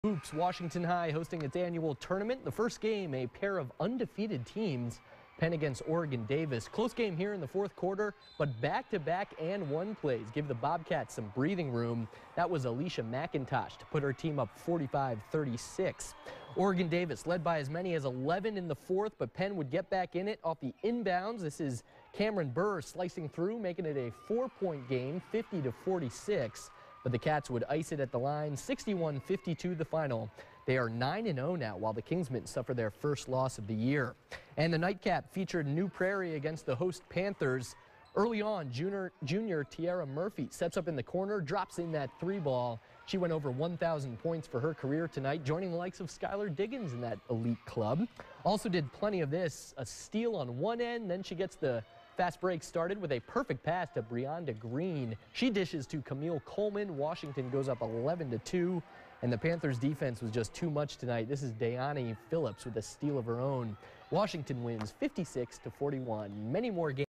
Holiday Hoops
SB Washington hosted day one of its annual basketball tournament. _________________